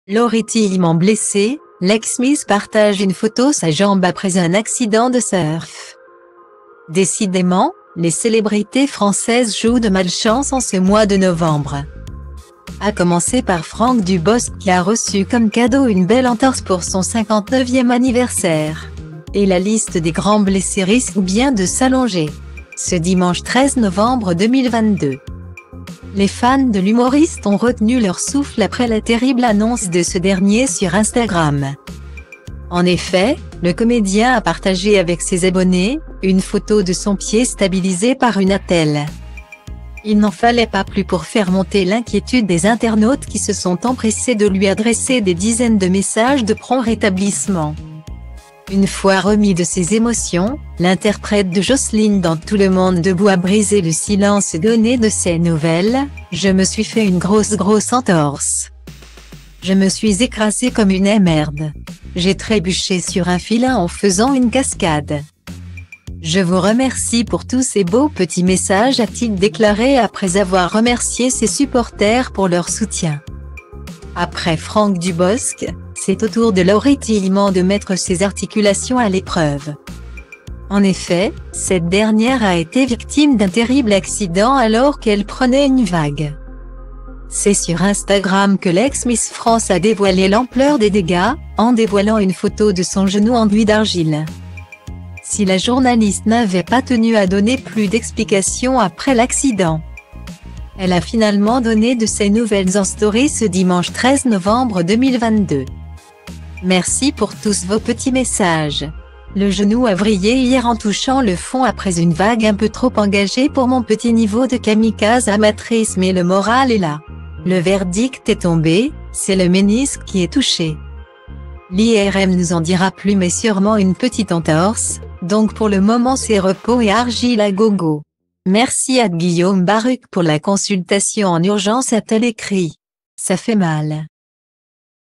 Laury Thilleman blessée : l’ex miss partage une photo sa jambe après un accident de surf
Décidément, les célébrités françaises jouent de malchance en ce mois de novembre.À commencer par Franck Dubosc qui a reçu comme cadeau une belle entorse pour son 59e anniversaire ! Et la liste des grands blessés risquent bien de s’allonger… Ce dimanche 13 novembre 2022,les fans de l’humoriste ont retenu leur souffle après la terrible annonce de ce dernier sur Instagram.